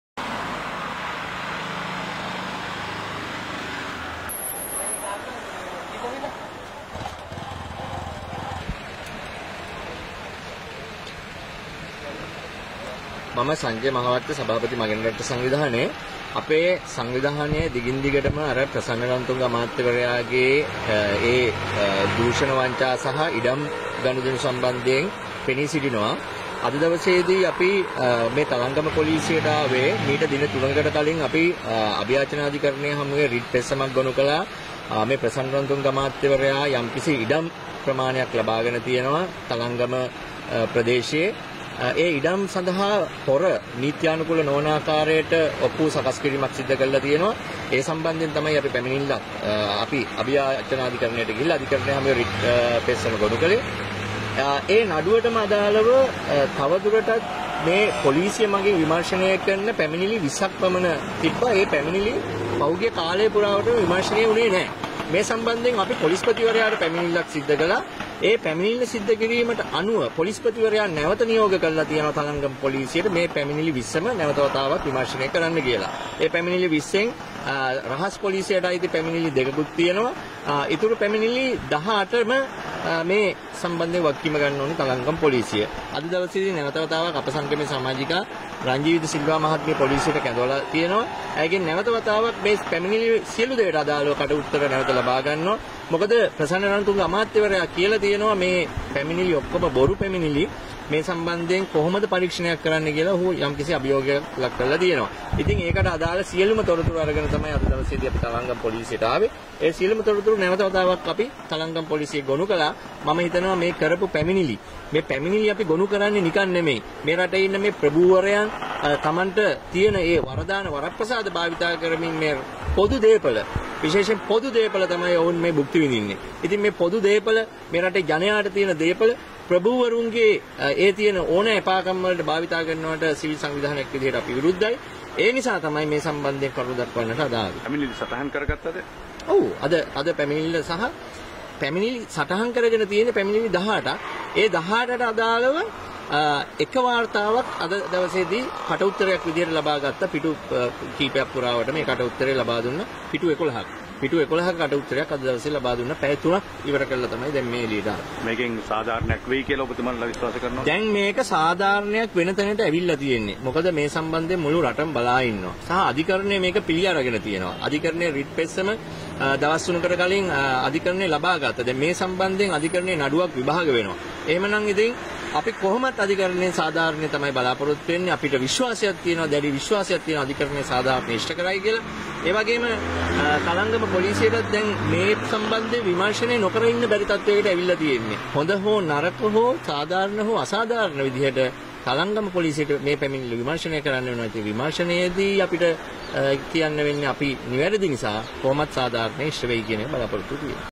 පොලිසිය ඇමති ප්‍රසන්නට විරුද්ධව පැමිණිලි 18 ක් විභාග කරල නෑ
ප්‍රසන්න රණතුංග ට එරෙහිව තලංගම පොලීසියේ කරන ලද පැමිණිලි 18 කට අදාලව විමර්ශණ නොකිරීම පිලිබදව පොලිස්පති වරයාට පැමිණිලි කිරීමෙන් අනතුරුව එම පැමිනිලි සියල්ලෙහිම විමර්ශනයක් කිරීම අද දින තලංගම පොලීසිය් ආරම්භ කලා.එහිදී පැමිණිලි කාරිය වූ අනිශ්කා රංජීවිද සිල්වා මහත්මිය සමග මගෙන් රටට සංවිධානයේ සභාපතිවරයා වරයාද තලංගම පොලීසියට පැමිණ තිබුනා